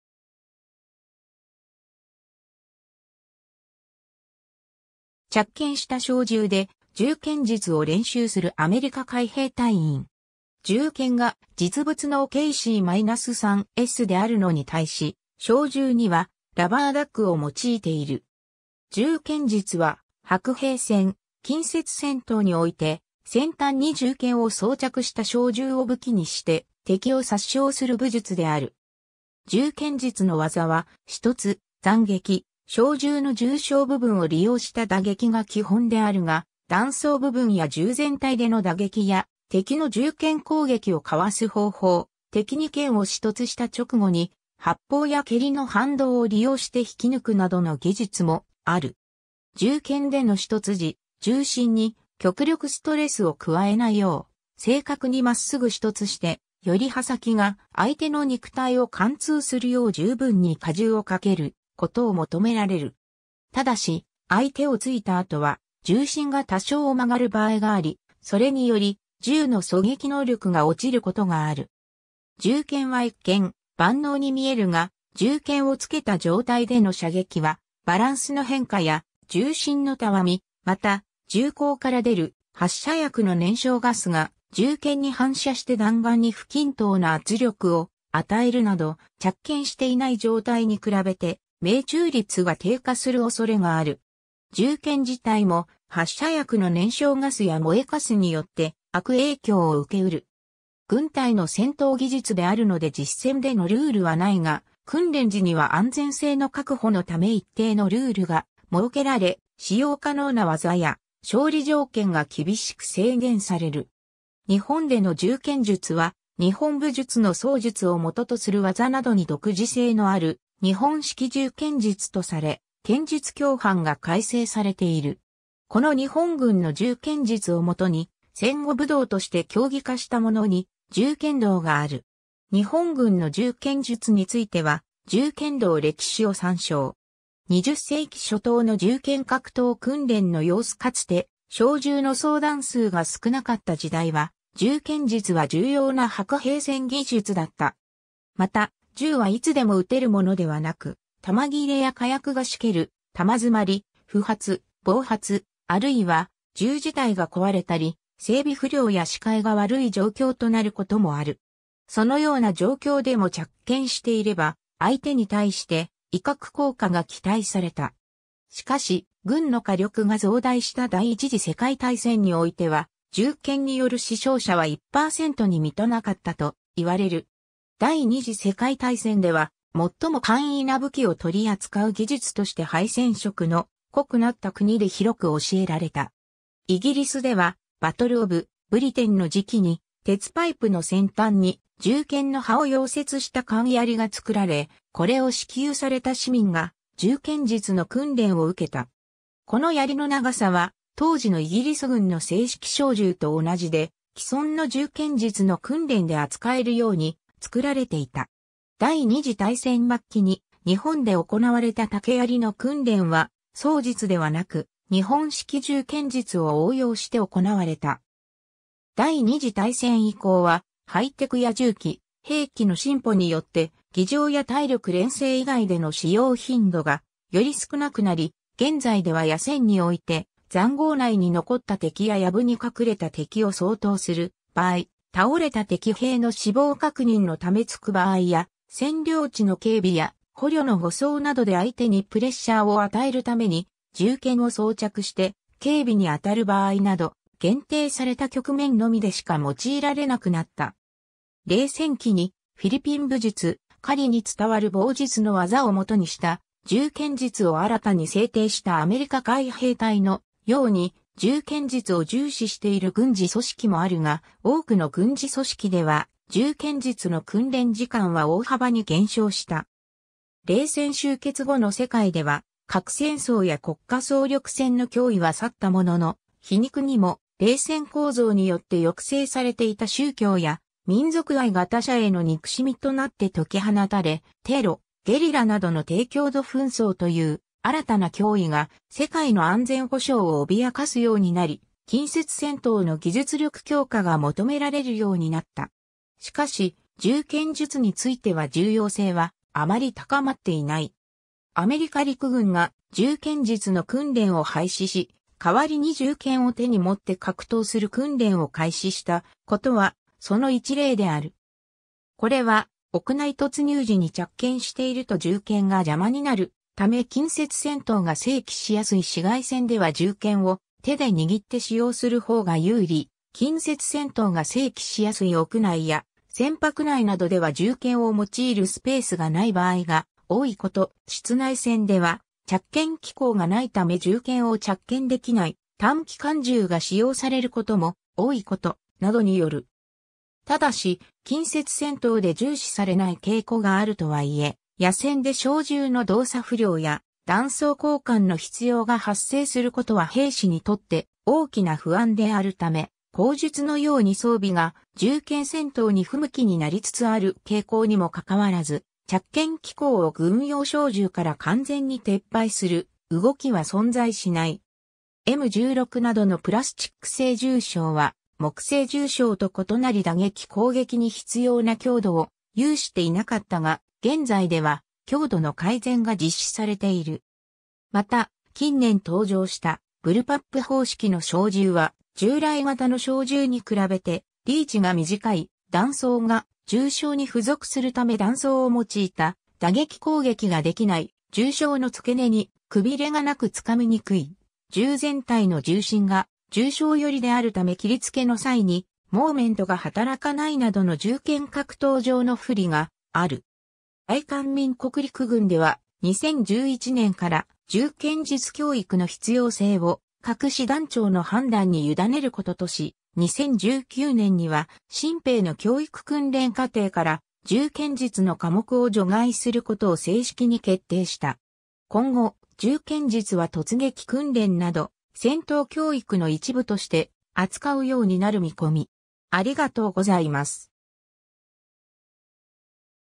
銃剣術
着剣した小銃で銃剣術を練習するアメリカ海兵隊員。
銃剣が実物のOKC-3Sであるのに対し、小銃にはラバーダック（訓練用模擬銃）を用いている。
 銃剣術（じゅうけんじゅつ）は、白兵戦・近接戦闘において、先端に銃剣を装着（着剣）した小銃を武器にして敵を殺傷する武術である。
 銃剣術の技は、刺突（突き）、斬撃（斬りつけ）、小銃の銃床（ストック、台尻）部分を利用した打撃が基本であるが、弾倉部分や銃全体での打撃や敵の銃剣攻撃をかわす方法、敵に剣を刺突した直後に発砲や蹴りの反動を利用して引き抜くなどの技術もある。
銃剣での刺突時、銃身に極力ストレスを加えないよう、正確にまっすぐ刺突して、より刃先が相手の肉体を貫通するよう十分に荷重をかけることを求められる。
ただし、相手を突いた後は、銃身が多少曲がる場合があり、それにより銃の狙撃能力が落ちることがある。
 銃剣は一見、万能に見えるが、銃剣を付けた状態での射撃はバランスの変化や銃身のたわみ、また、銃口から出る発射薬の燃焼ガスが銃剣に反射して弾丸に不均等な圧力を与えるなど、着剣していない状態に比べて命中率が低下するおそれがある。
銃剣自体も、発射薬の燃焼ガスや燃えカスによって悪影響を受け得る。
 軍隊の戦闘技術であるので実戦でのルールは無いが、訓練時には安全性の確保のため一定のルールが設けられ、使用可能な技や勝利条件が厳しく制限される。
 日本での銃剣術は、日本武術の槍術をもととする技などに独自性のある日本式銃剣術とされ、剣術教範が改正されている。
この日本軍の銃剣術を元に、戦後武道として競技化したものに銃剣道がある。
 日本軍の銃剣術については、銃剣道#歴史を参照。
 20世紀初頭の銃剣格闘訓練の様子（日本陸軍、1903年頃） かつて小銃の装弾数が少なかった時代は、銃剣術は重要な白兵戦技術だった。
また、銃はいつでも撃てるものではなく、弾切れや火薬が湿気る、弾詰まり、不発、暴発、あるいは銃自体が壊れたり、整備不良や視界が悪い状況となることもある。
そのような状況でも着剣していれば相手に対して威嚇効果が期待された。
しかし、軍の火力が増大した第一次世界大戦においては、銃剣による死傷者は1パーセントに満たなかったと言われる。
 第二次世界大戦では、最も簡易な武器を取り扱う技術として敗戦色の濃くなった国で広く教えられた。
イギリスではバトル・オブ・ブリテンの時期に鉄パイプの先端に銃剣の刃を溶接した簡易槍が作られ、これを支給された市民が銃剣術の訓練を受けた。
この槍の長さは当時のイギリス軍の正式小銃と同じで、既存の銃剣術の訓練で扱えるように作られていた。
第二次大戦末期に日本で行われた竹槍の訓練は、槍術ではなく日本式銃剣術を応用して行われた。
 第二次大戦以降は、ハイテクや銃器、兵器の進歩によって、儀仗や体力練成以外での使用頻度がより少なくなり、現在では野戦において塹壕内に残った敵や藪に隠れた敵を掃討する場合、倒れた敵兵の死亡確認のため突く場合や、占領地の警備や捕虜の護送などで相手にプレッシャーを与えるために銃剣を装着して警備に当たる場合など、限定された局面のみでしか用いられなくなった。
冷戦期に、フィリピン武術「カリ」に伝わる棒術の技をもとにした銃剣術を新たに制定したアメリカ海兵隊のように、銃剣術を重視している軍事組織もあるが、多くの軍事組織では銃剣術の訓練時間は大幅に減少した。
 冷戦終結後の世界では、核戦争や国家総力戦の脅威は去ったものの、皮肉にも冷戦構造によって抑制されていた宗教や民族愛が他者への憎しみとなって解き放たれ、テロ、ゲリラなどの低強度紛争という新たな脅威が世界の安全保障を脅かすようになり、近接戦闘の技術力強化が求められるようになった。
しかし、銃剣術については重要性はあまり高まっていない。
アメリカ陸軍が銃剣術の訓練を廃止し、代わりに銃剣を手に持って格闘する訓練を開始したことは、その一例である。
これは、屋内突入時に着剣していると銃剣が邪魔になるため近接戦闘が生起しやすい市街戦では銃剣を手で握って使用するほうが有利、近接戦闘が生起しやすい屋内や船舶内などでは銃剣を用いるスペースが無い場合が多いこと、室内戦では着剣機構が無いため銃剣を着剣できない短機関銃（サブマシンガン）が使用されることも多いこと、などによる。
 ただし、近接戦闘で重視されない傾向があるとはいえ、野戦で小銃の動作不良（ジャム）や弾倉交換の必要が発生することは兵士にとって大きな不安であるため、後述のように装備が銃剣戦闘に不向きになりつつある傾向...